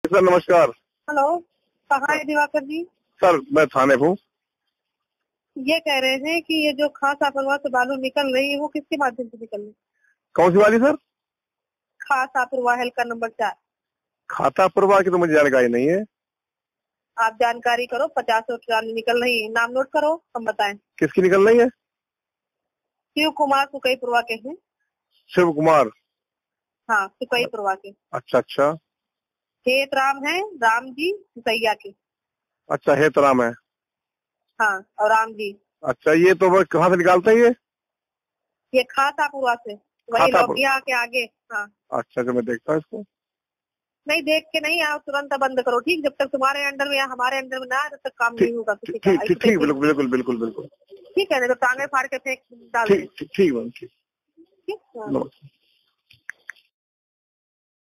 बीजेपी विधायक ने इंस्पेक्टर से कहा, बालू निकलेगी तो हमारे आर्डर से,नहीं तो टांगे फाड़ दो
लखीमपुर खीरी: उत्तर प्रदेश के लखीमपुर खीरी जिले में श्रीनगर विधानसभा से बीजेपी विधायक मंजू त्यागी का एक और ऑडियो वायरल हो गया है. शनिवार को फूलबेहड़ कोतवाली के इन्स्पेक्टर दिवाकर को फोन पर जूतों से मारने की धमकी दी थी और आज अवैध खनन को इंस्पेक्टर के साथ बातचीत का ऑडिया वायरल हो रहा है.